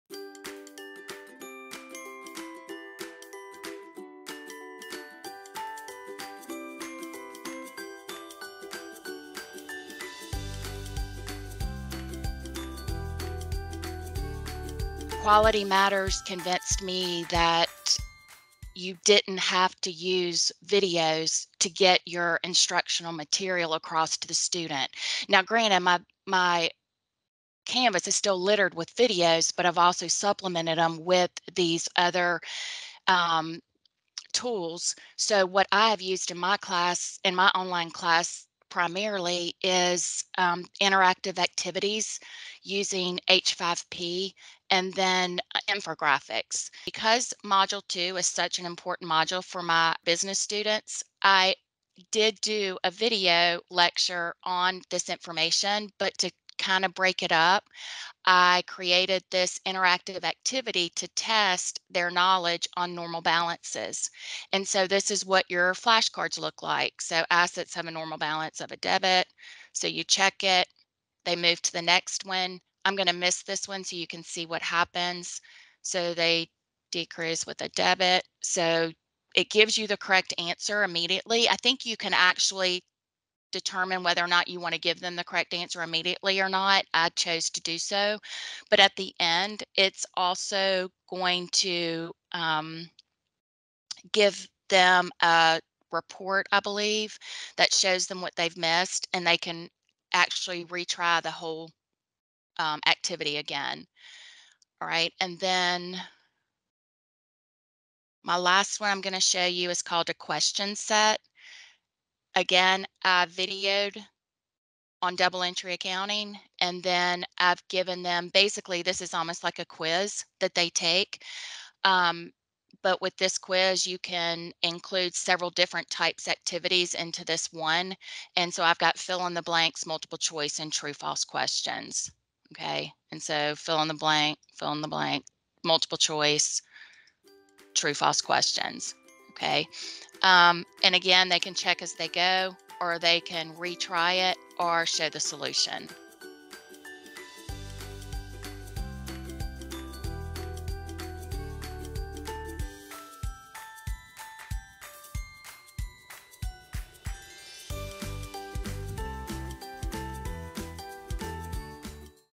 3-2-1 Video - Debra Schisler
In this 3-2-1 video, Debra Schisler describes some tools she uses to represent content in multiple ways in her online classes.